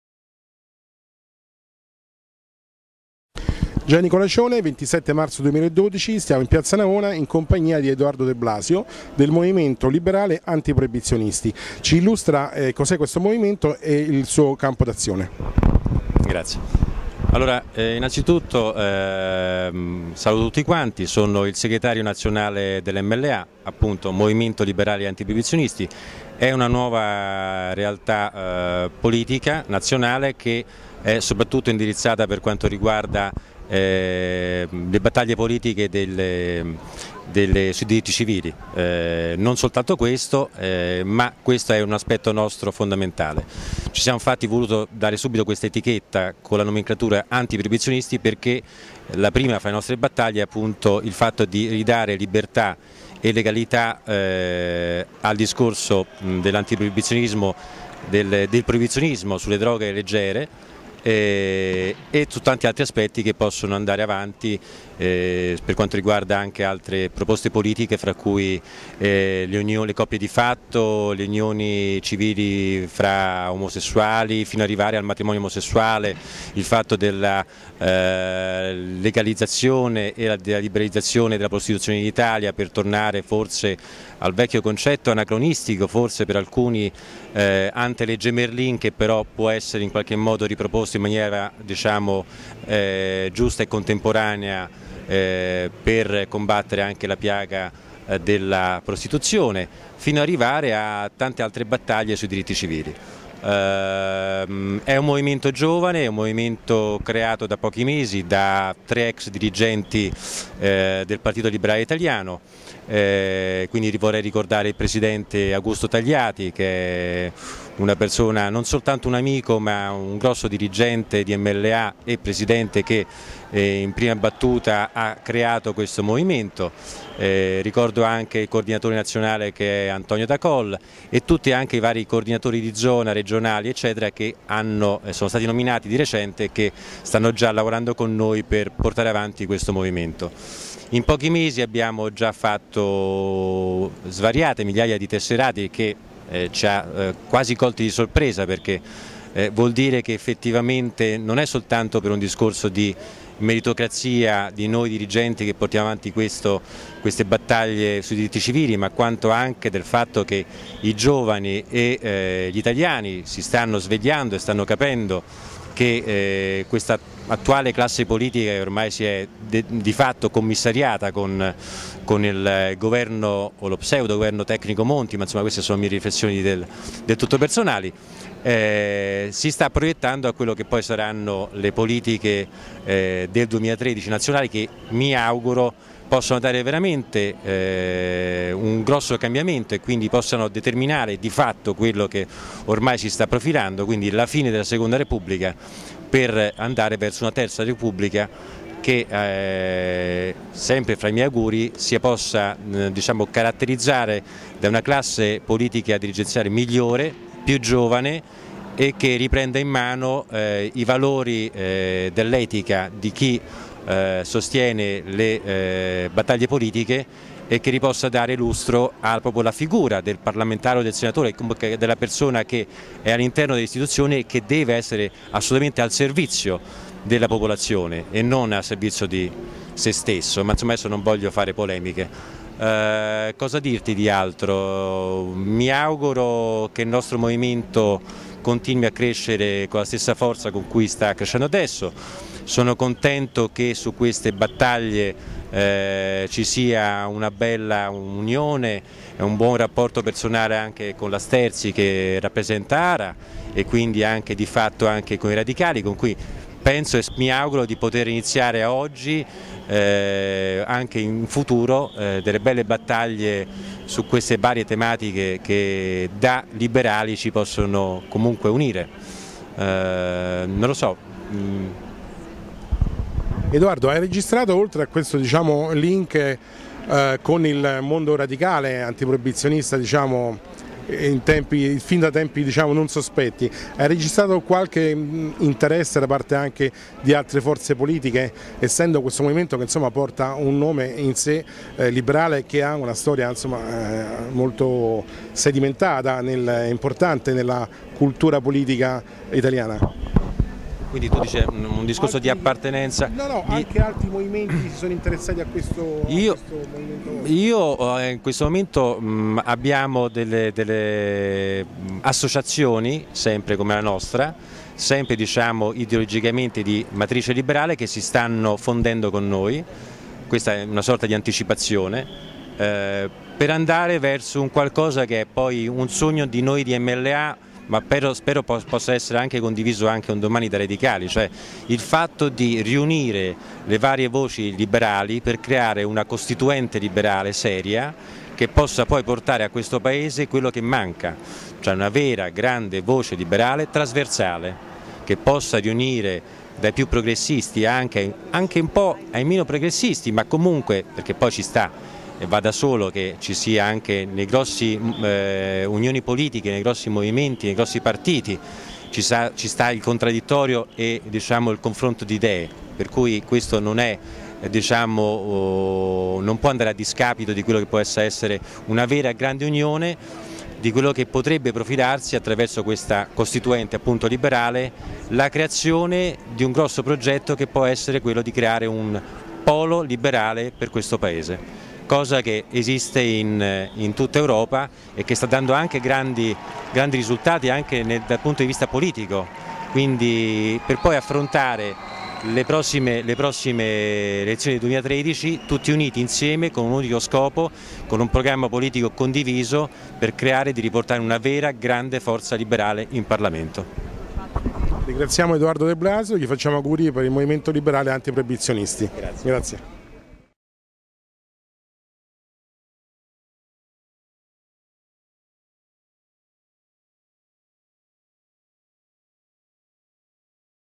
Edoardo de Blasio segretario MLA - Movimento Liberali Antiproibizionisti "Chi siamo!"
- Intervista ad Edoardo de Blasio segretario del MLA -- Movimento Liberali Antiproibizionisti a margine della conferenza stampa tenutasi il 27 marzo 2012 presso il Senato della Repubblica avente come tema "Effetti Collaterali della Fini-Giovanardi".